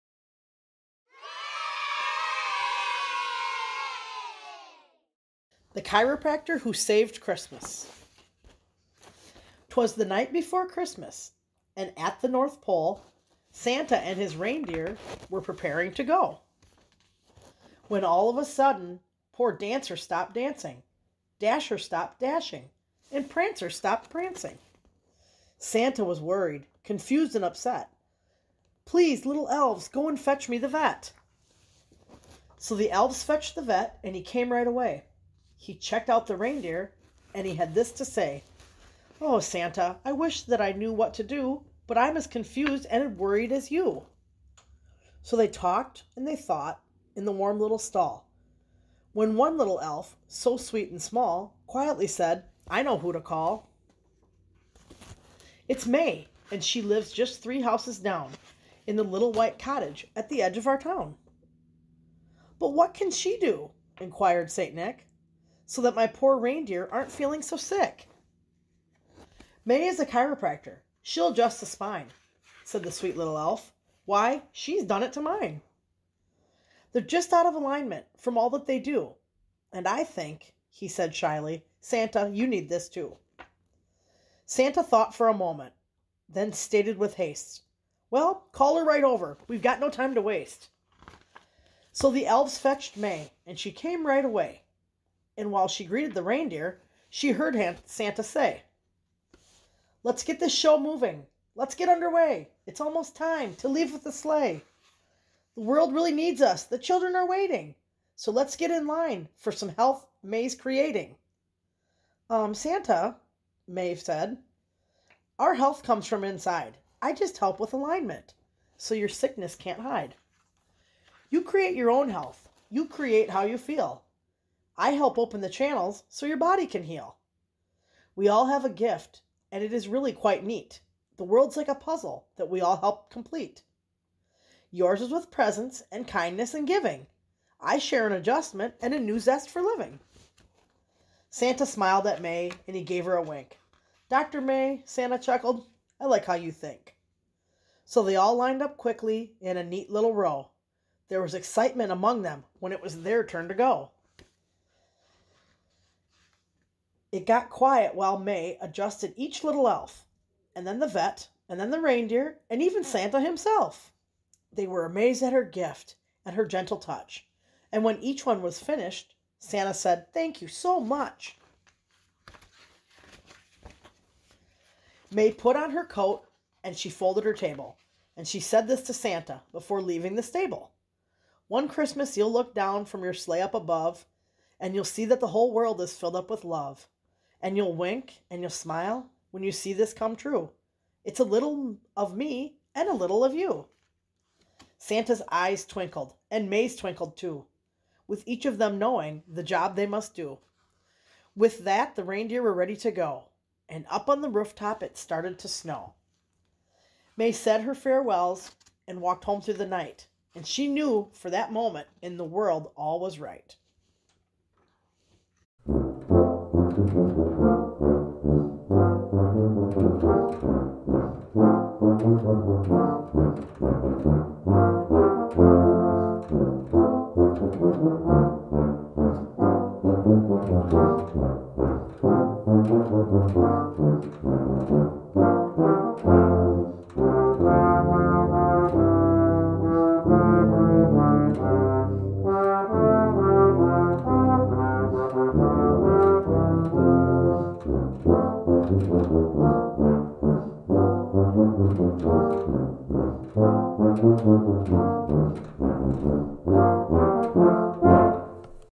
The ChiropractorWho Saved Christmas
Narrated by Cher Venden
Story Time
w/ Zwingli UCC
Monticello, WI